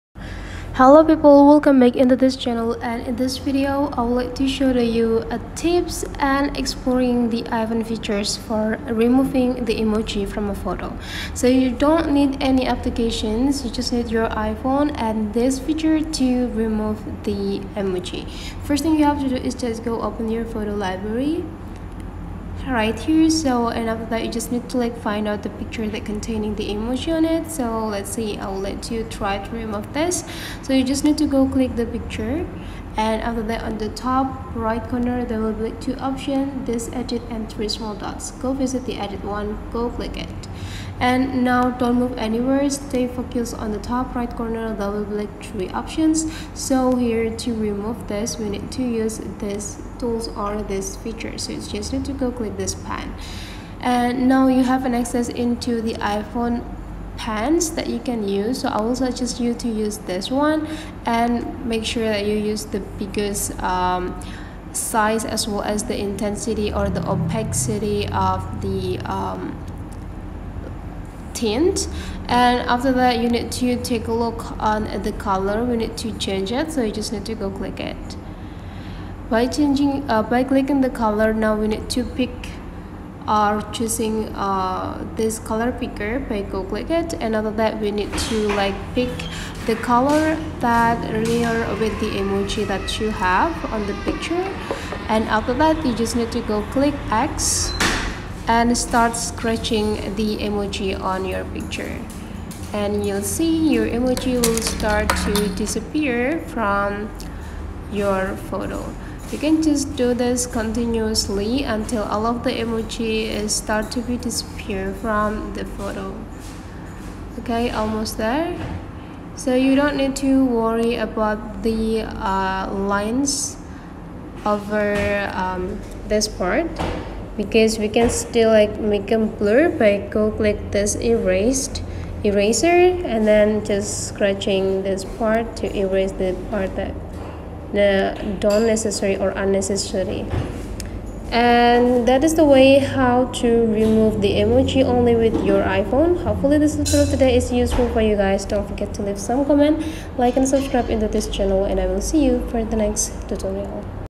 ~ How to Remove Emojis from Photos Using Only Your iPhone! 📱✨
In this tutorial, discover the magic of removing emojis from your photos using just your iPhone! Are you tired of emojis cluttering up your favorite pictures? Well, fret no more because I'm about to reveal a simple yet effective method to eliminate those pesky emojis with ease.

Imagine having the power to erase emojis from your cherished memories effortlessly. With the step-by-step guide provided in this video, you'll unlock the secrets of your iPhone's editing capabilities, transforming your photos into emoji-free masterpieces.

Firstly, we'll delve into the essential tools and features within your iPhone's photo editing options. No need for third-party apps or complicated software – everything you need is right at your fingertips. From selecting the appropriate brush size to adjusting intensity, you'll learn how to tailor the editing process to suit your specific needs.

Next, we'll tackle the removal process itself. Follow along as we demonstrate the exact steps to take, from identifying emojis in your photos to meticulously erasing them without leaving a trace. Say goodbye to those unwanted emojis as we guide you through the process seamlessly.

But that's not all! We'll also share some pro tips to enhance your editing skills further. Whether it's blending colors seamlessly or refining edges for a flawless finish, you'll walk away with newfound expertise in photo editing on your iPhone.

Let's dive in and unlock the full potential of your iPhone's editing capabilities together!